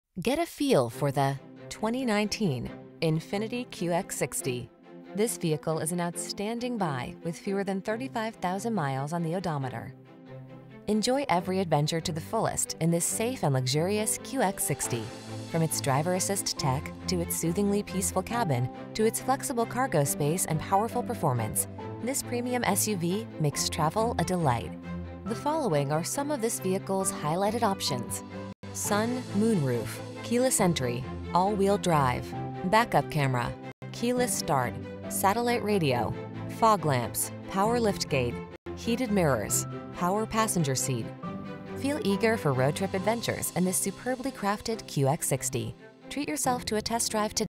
2019 INFINITI QX60 Massapequa, Huntington, Hempstead, Long Island, Nassau NY 11602
Liquid Platinum [LISTING TYPE] 2019 INFINITI QX60   available in Massapequa, New York at Infiniti of Massapequa. Servicing the Huntington, Hempstead, Long Island, Nassau NY area.

KBB.com 5-Year Cost to Own Awards. Only 30,205 Miles! Boasts 26 Highway MPG and 19 City MPG!